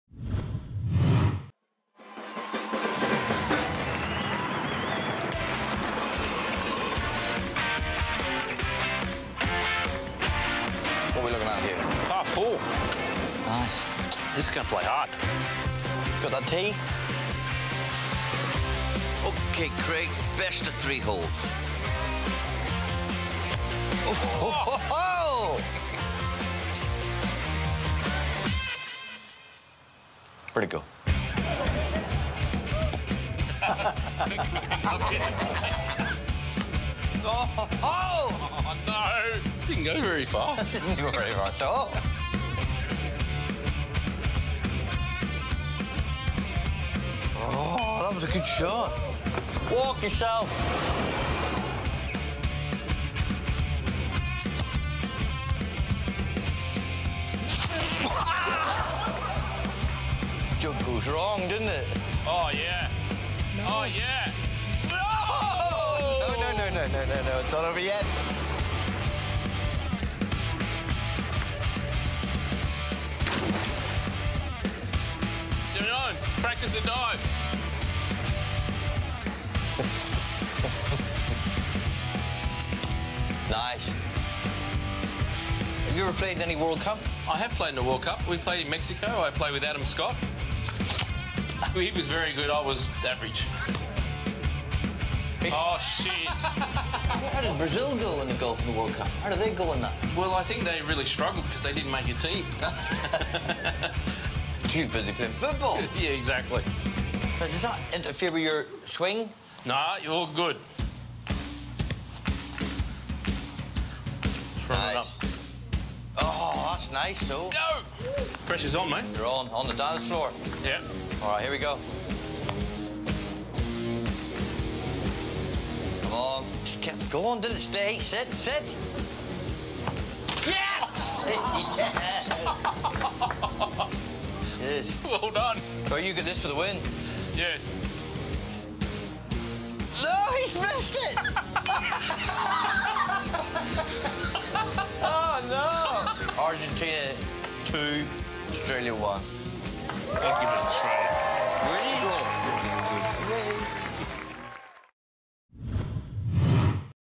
Jimeoin vs Golf Legend Craig Parry
Jimeoin takes on Australian Golfing legend Craig Parry in an interesting game of "Foot Golf".